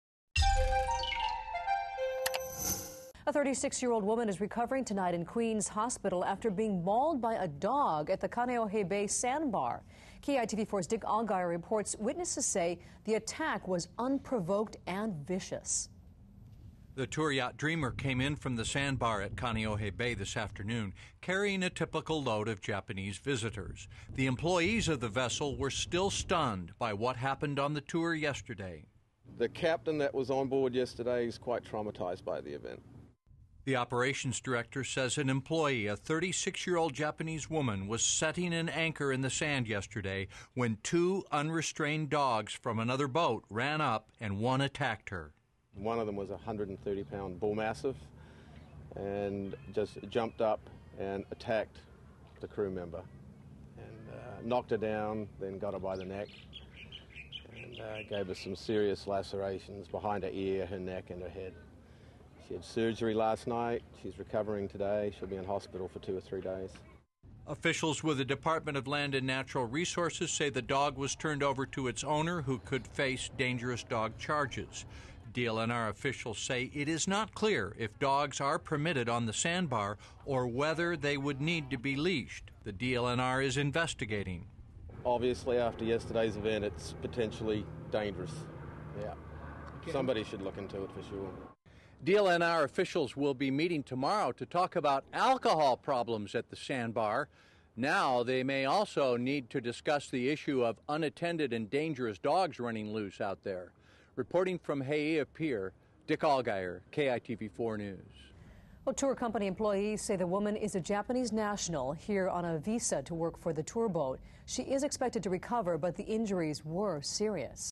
Dog Attacks Woman At Kaneohe Sandbar
A 36-year-old woman is recovering in Queen's hospital after being mauled by a dog at the Kaneohe Bay sandbar. For news you need to know,